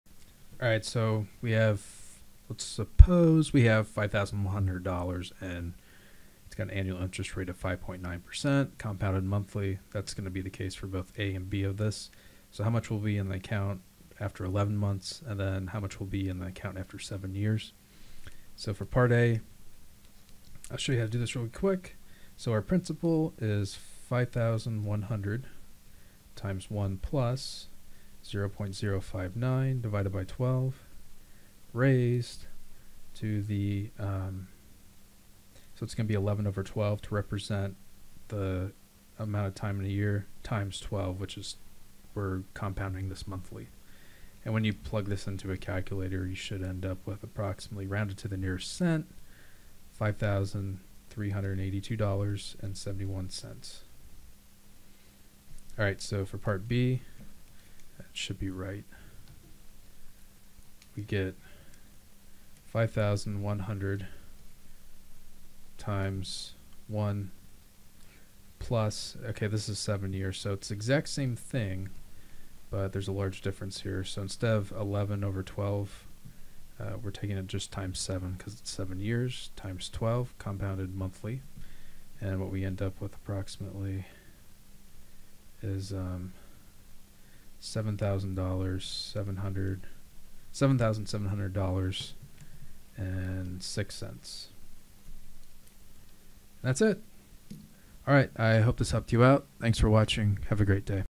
Finance. Suppose that $5,100 is invested at 5.9% annual interest rate, compounded monthly.
Finance. Suppose that $5,100 is invested at 5.9% annual interest rate, compounded monthly. How much money will be in the account in (A) 11 months? (B) 7 years?